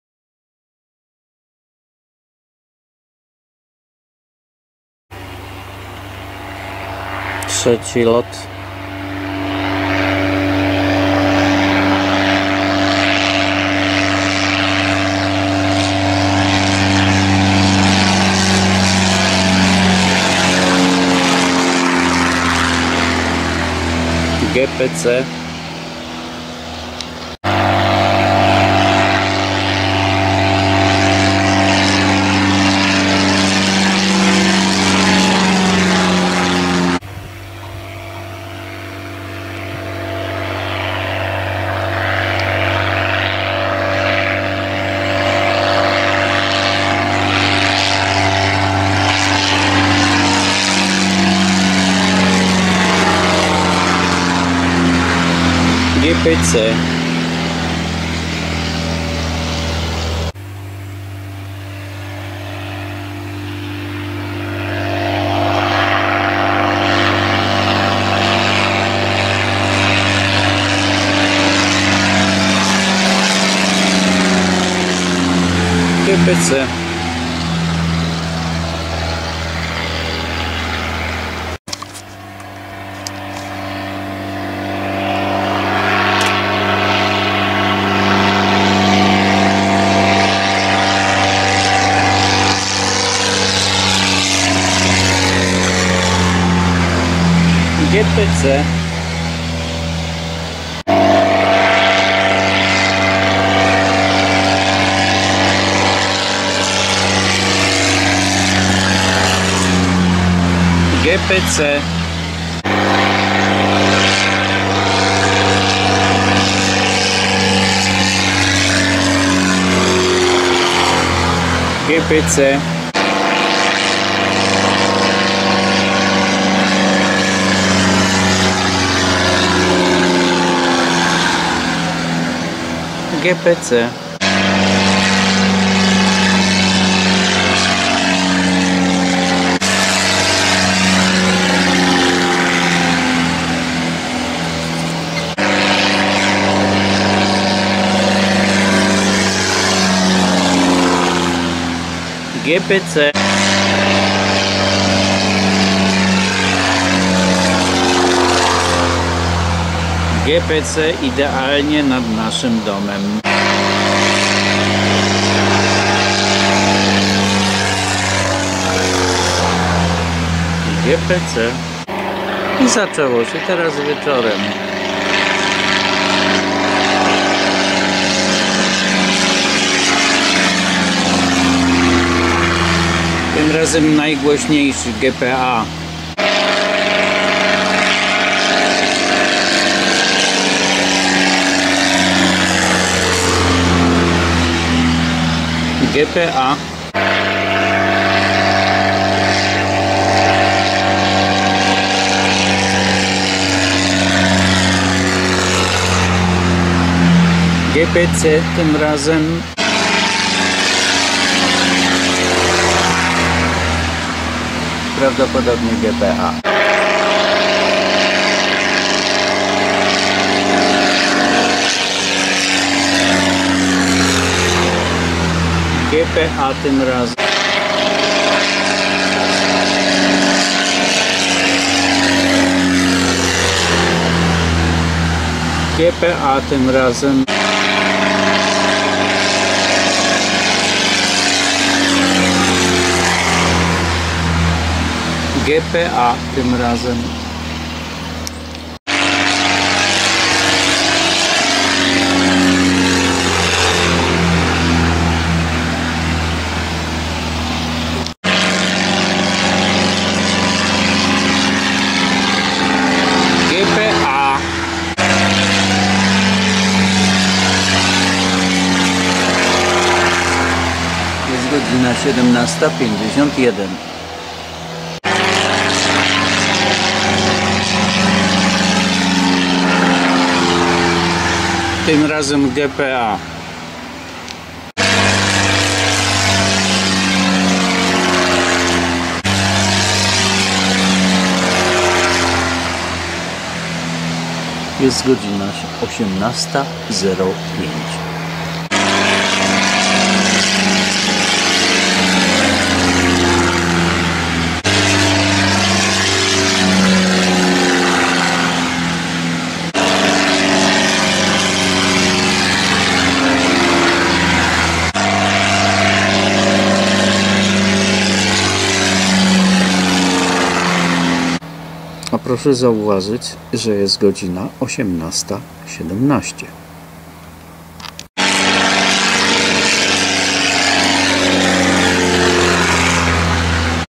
Areoklub Bydgoski dalej zakłóca spokój mieszkańcom dzielnicy!!!
Filmik pokazuje jak można mając kasę uprzykrzać innym ludziom normalne życie.